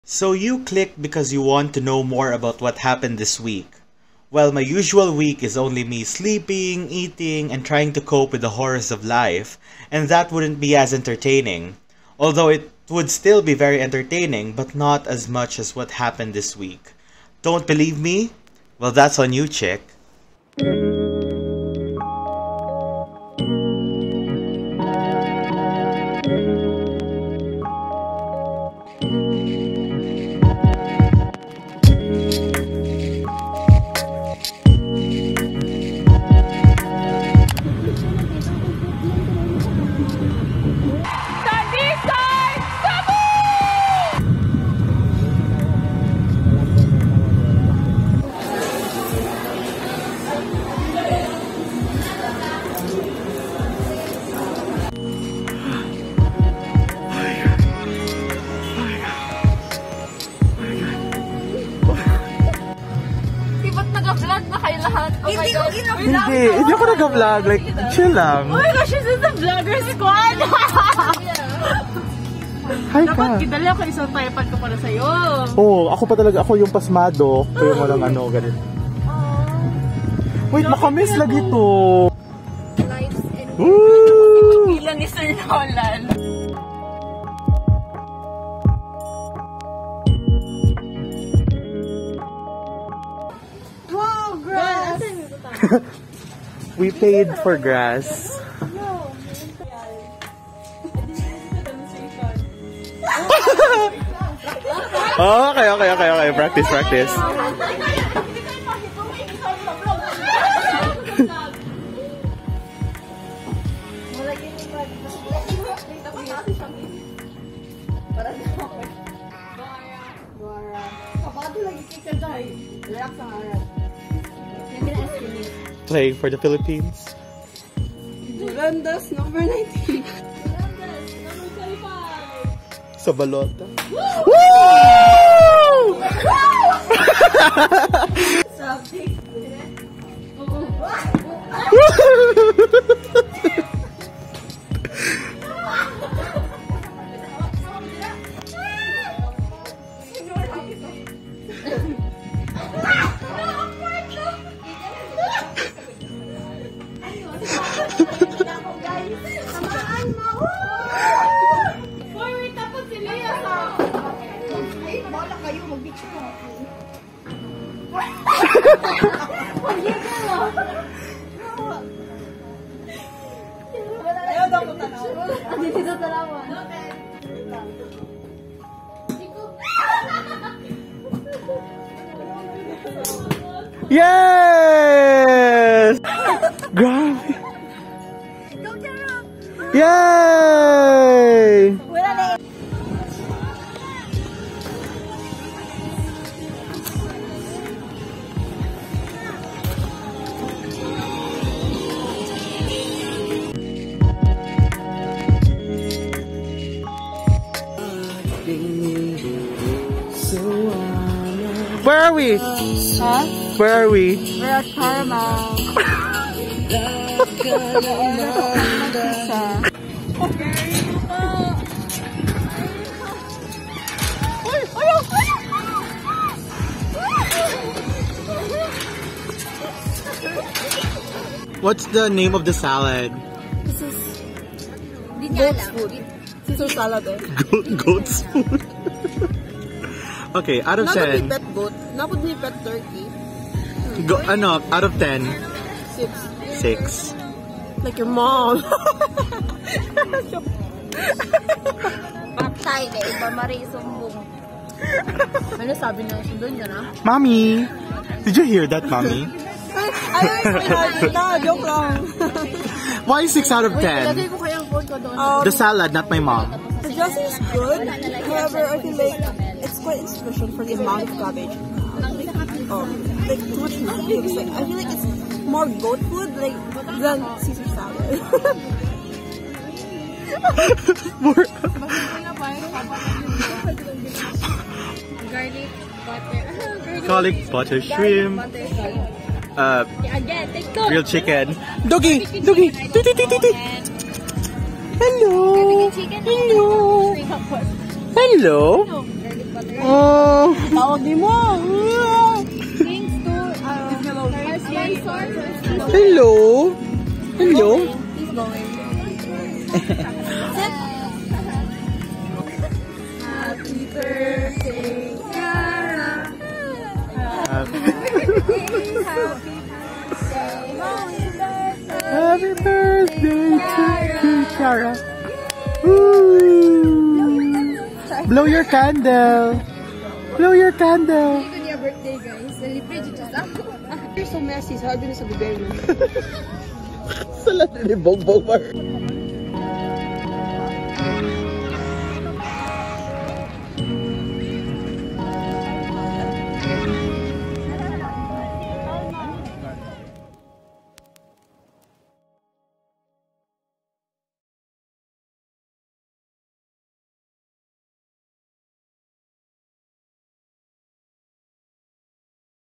Fugly Golfing | esKapade E1
Fugly Golfing with friends! I've never tried golf before but hey, there's always a first for everything.